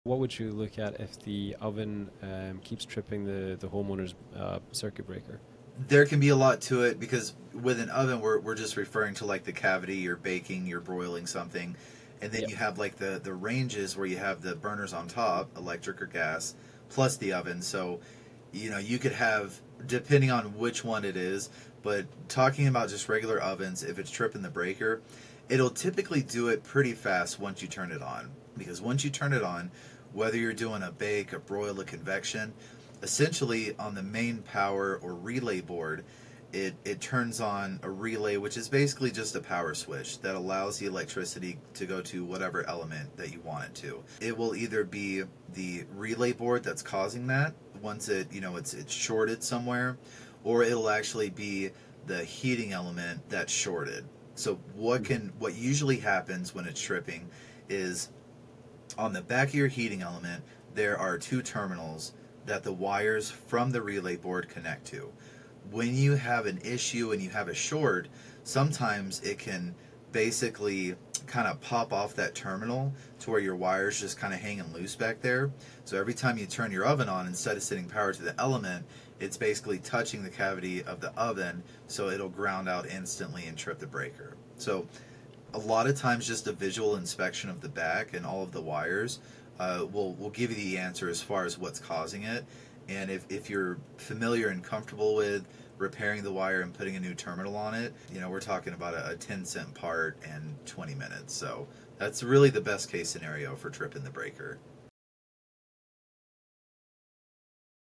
Solving An Oven That Trips Your Breaker
For a full written guide on how to fix an oven that's tripping a circuit breaker, make sure to check out:

This is just a short clip from our longer conversation on ovens. Where we cover much more information on maintenance, troubleshooting, and the best brands. Be sure to check it out!